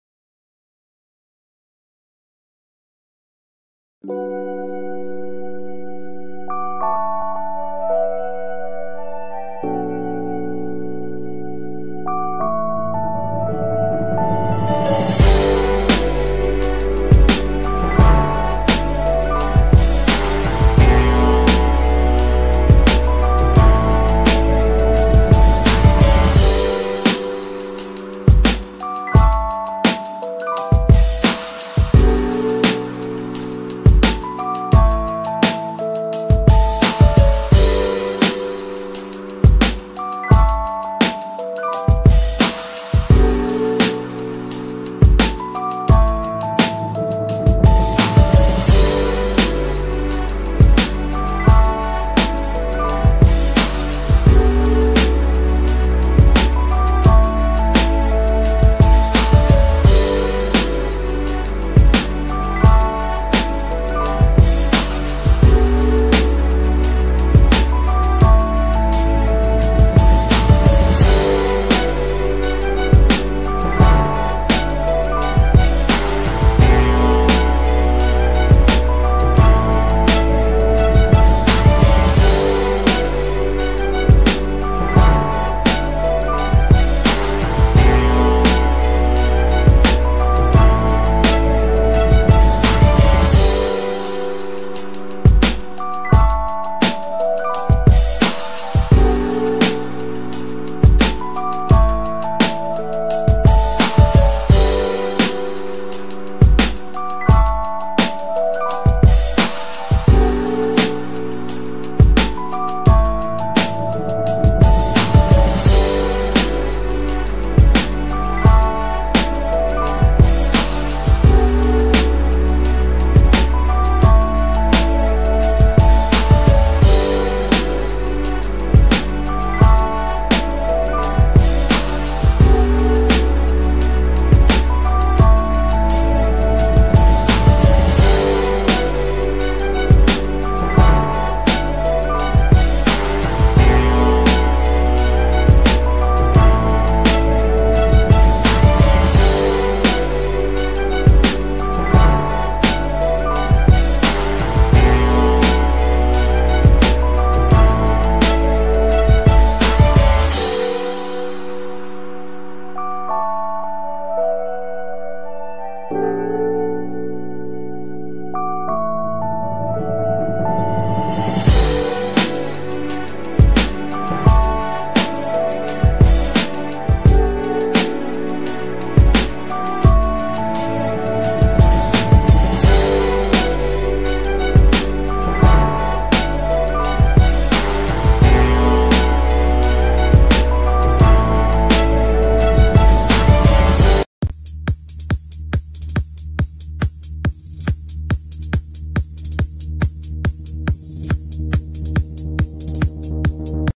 Cara de la hoyde and nathan massey spotted with one-day-old son | News TODAY
CARA De La Hoyde and her boyfriend Nathan Massey have pictured for the first time since becoming parents. The Love Island couple welcomed their baby son on Wednesday and Cara excitedly told their fans...